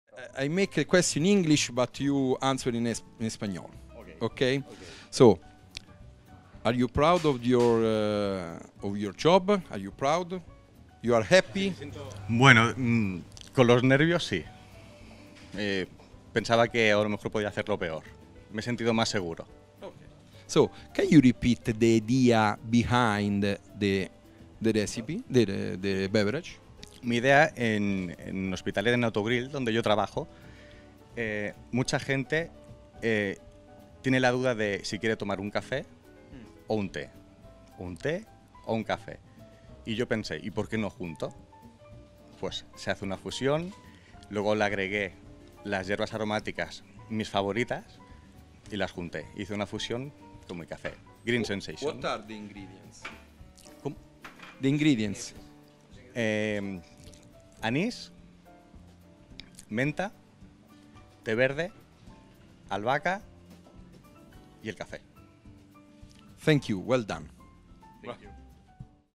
Entrevista a Felipe Escodar en el Campeonato Europeo de Baristas 2016
El español Felipe Escodar ha ganado el Campeonato Europeo de Baristas 2016, organizado por el Grupo Autogrill y Kimbo Café.